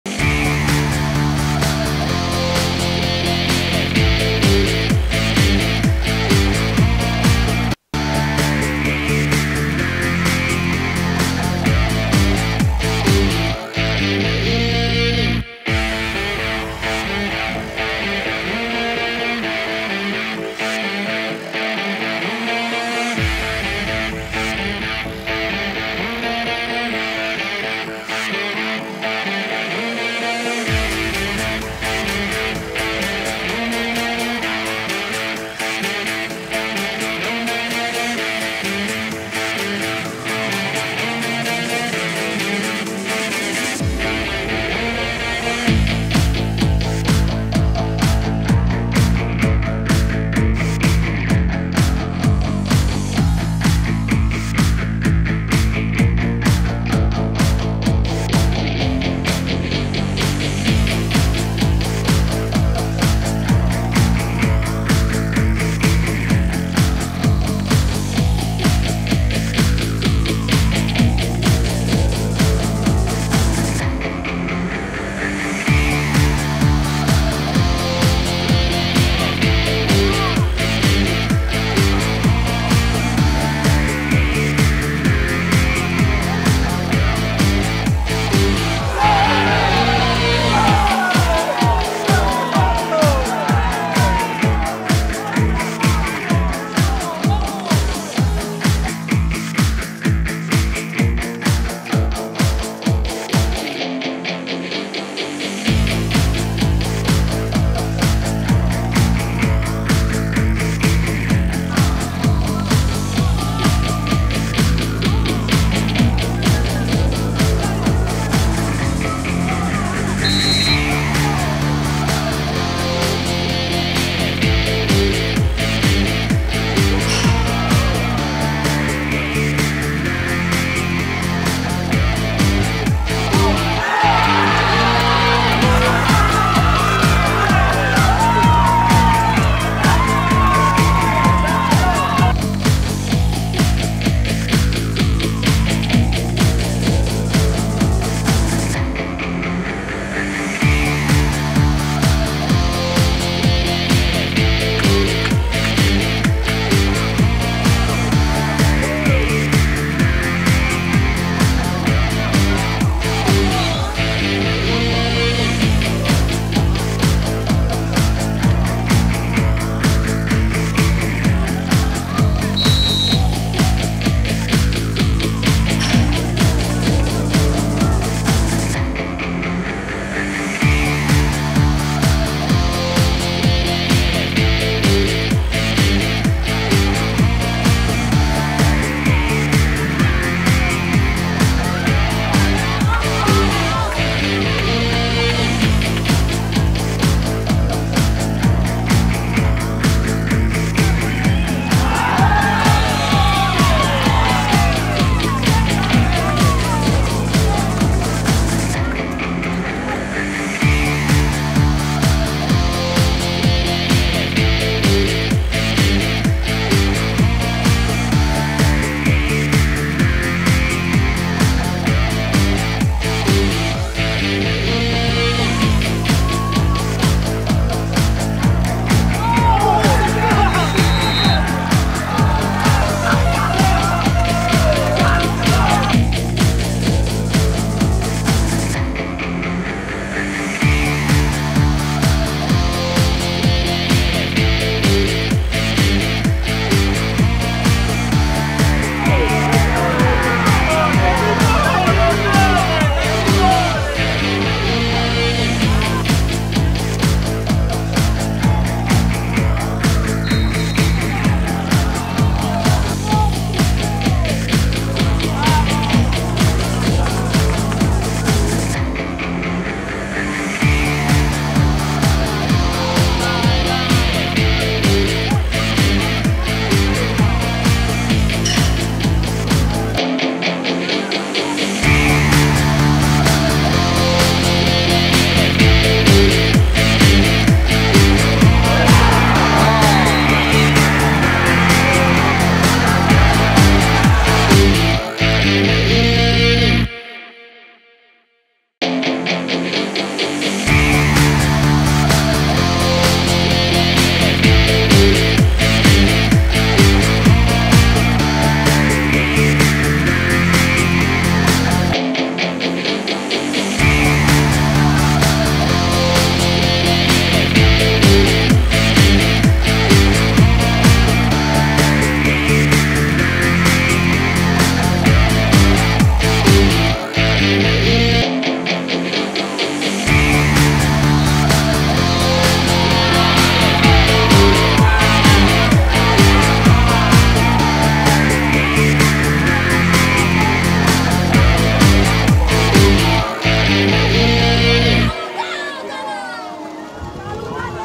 U17 Highlights Daken Invicta Matera - Policoro
Campionato regionale della Basilicata Under 17 stagione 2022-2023.
Highlights della gara Daken Invicta Matera - Policoro
Risultato finale 3-2 a favore della Daken Invicta Matera.